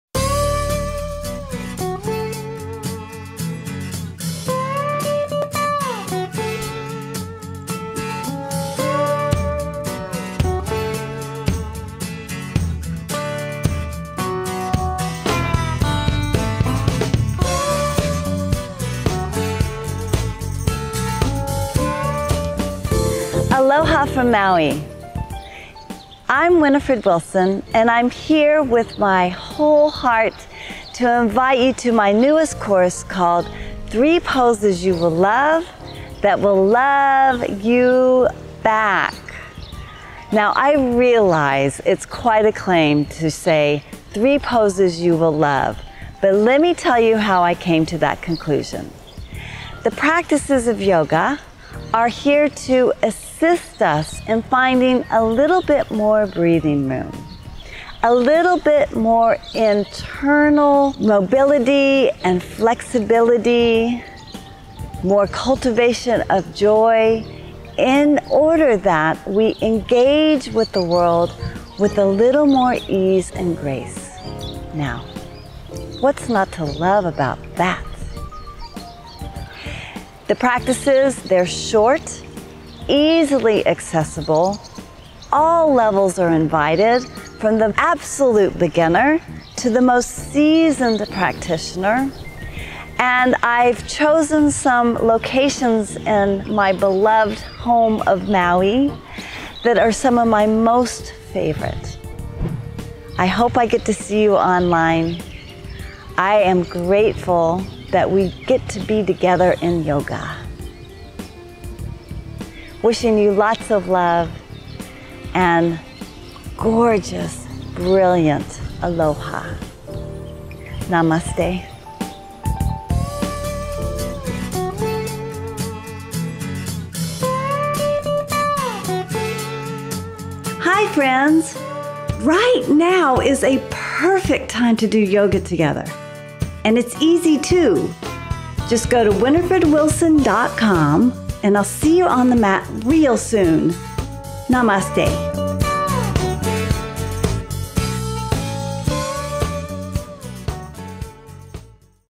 Cultivate Joy: An Invitation for YOU!
Three Poses That You Will Love:
You are invited to join Winifred to dive into cultivating joy! In this course consisting of six short yoga practices, we will explore three poses in such a way that will expand your mindset as well as your heart. This is an offering for all levels of practice, and anyone interested in deepening their breath and lightening their mood! Each of the short, digestible segments are sure to leave you feeling balanced, centered and full of joy!